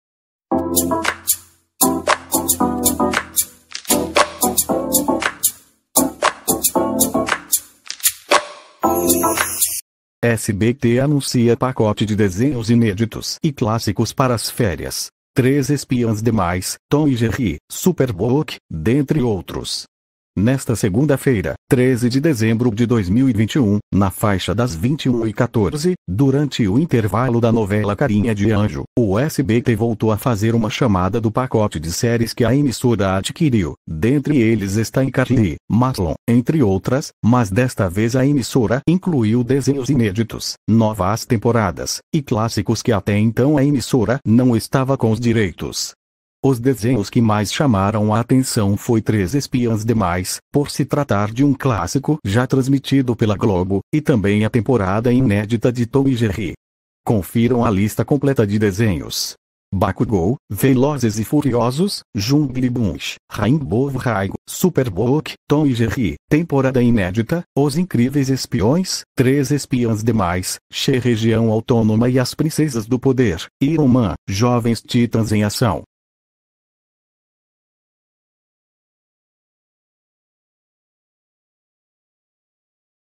💣💥ALÉM DO PACOTÃO DE SÉRIES, SBT ANUNCIA PACOTÃO DE NOVOS DESENHOS PARA AS FÉRIAS!!!💥📸
SBT anuncia pacote de desenhos inéditos e clássicos para as férias; Três Espiãs Demais, Tom e Jerry, Superbook, dentre outros.

Nesta segunda-feira (13/12/2021), na faixa das 21h14, durante o intervalo da novela Carinha de Anjo, o SBT voltou a fazer uma chamada do pacote de séries que a emissora adquiriu, dentre eles está Icarly, Marlon, entre outras, mas desta vez a emissora incluiu desenhos inéditos, novas temporadas, e clássicos que até então a emissora não estava com os direitos.

Os desenhos que mais chamaram a atenção foi Três Espiãs Demais, por se tratar de um clássico já transmitido pela Globo, e também a temporada inédita de Tom e Jerry.

Confiram a lista completa de desenhos:

– Bakugou

– Velozes e Furiosos

– Jungle Bunch

– Rainbow High

– Superbook

– Tom e Jerry (temporada inédita)

– Os Incríveis Espiões

– Três Espiãs Demais

– She-Ra e as Princesas do Poder

– Iron Man

– Jovens Titãs em ação